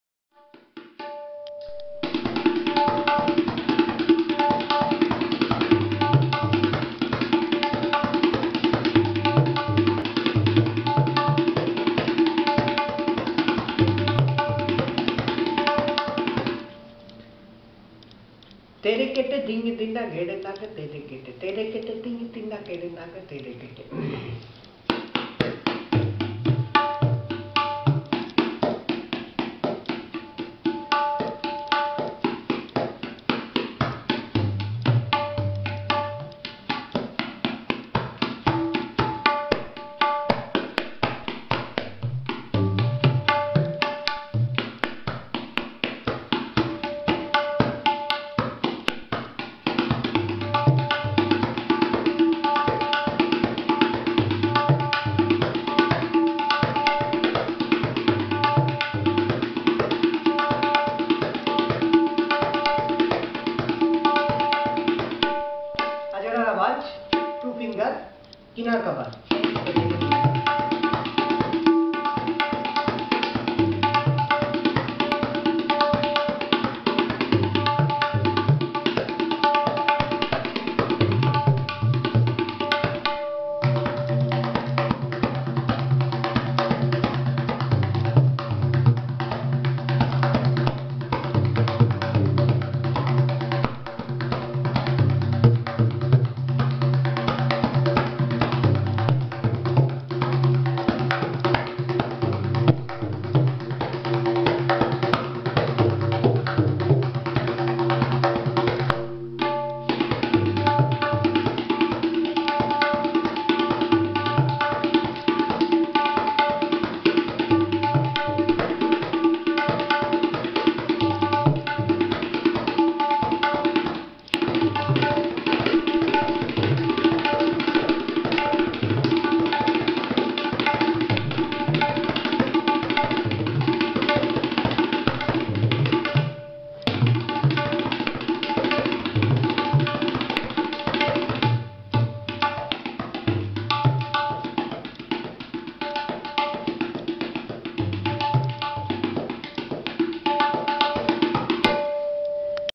Ajrara Gharana Rela 3 palta 4 Sumit Datta
Sumit Datta is a researcher in the field of Classical Indian Percussion. In this video he shares Ajrara Gharana Rela 3 palta 4